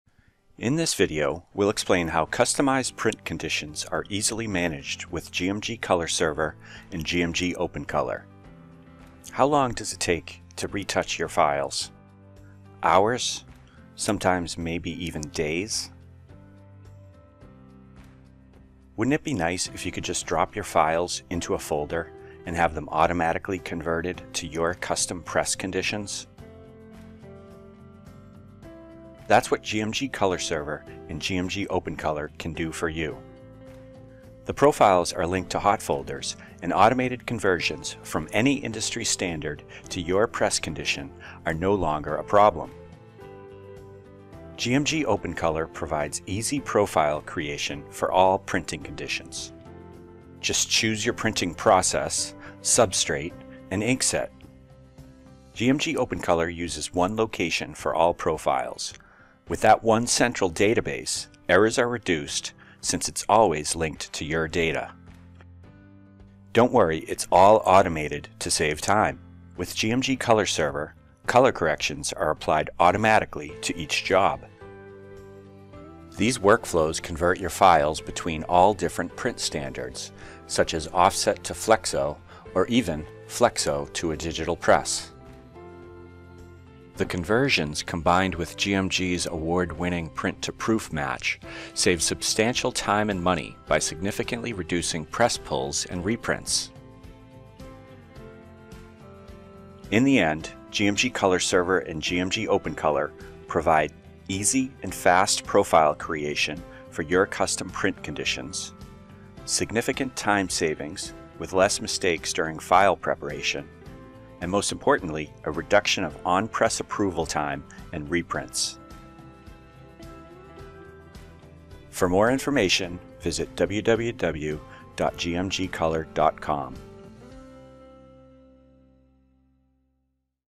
Customized Printing Conditions Made Easy with GMG ColorServer and GMG OpenColor
Learn how to easily manage customized print conditions with GMG ColorServer and GMG OpenColor.
Every press prints different. Your customer wants to match an industry standard. Here we explain how to make your press look like any industry standard of your choosing.